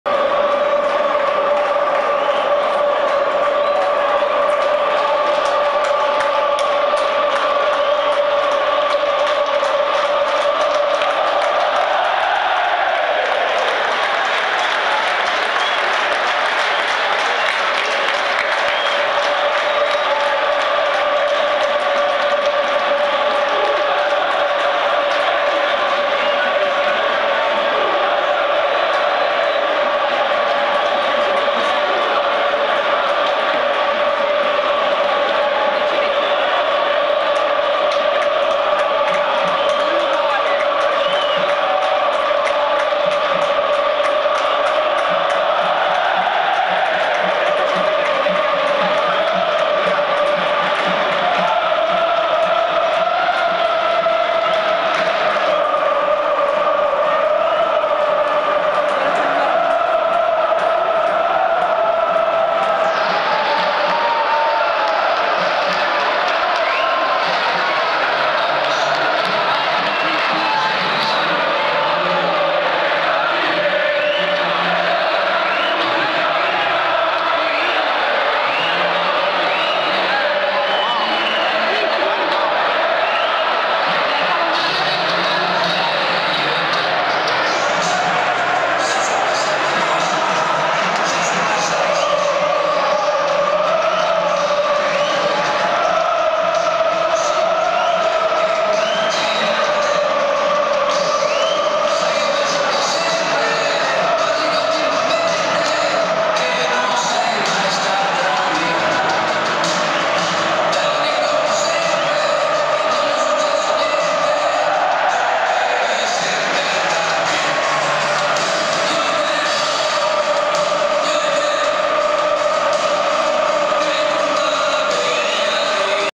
2018/19 Parma - ATALANTA, Serie A
2018/19 Parma - ATALANTA, Serie A
I festeggiamenti dei nerazzurri sotto lo spicchio riservato ai propri tifosi
Video: Giovanni Padovani per Sportpeople.net